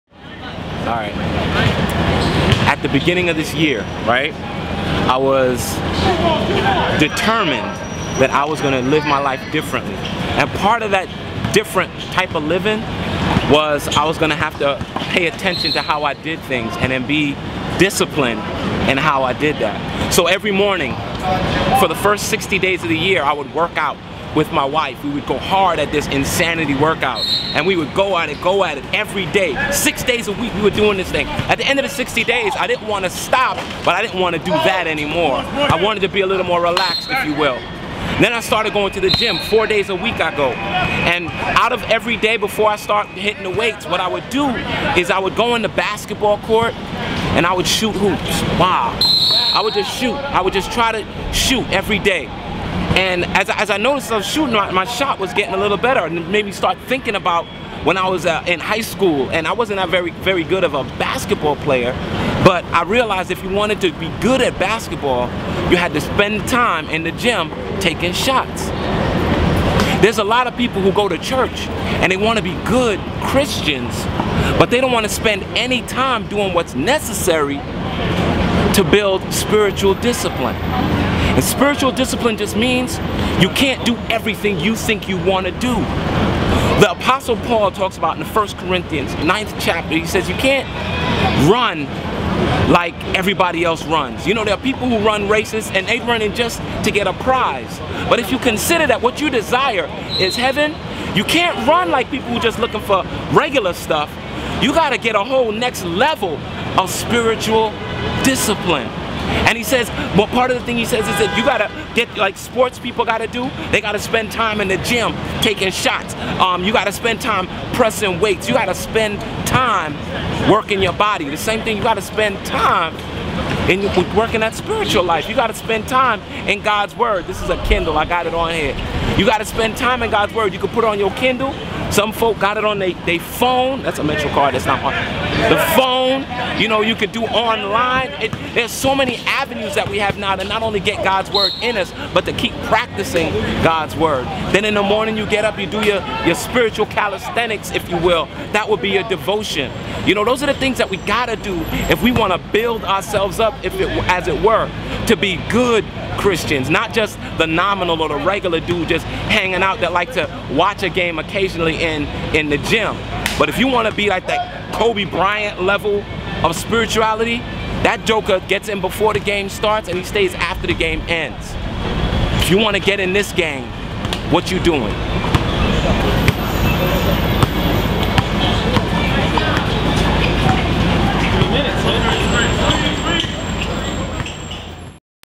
Discipline
Pastor Delroy Books discusses spiritual discipline. We know we need discipline to excel on the basketball court and the football field. What about life?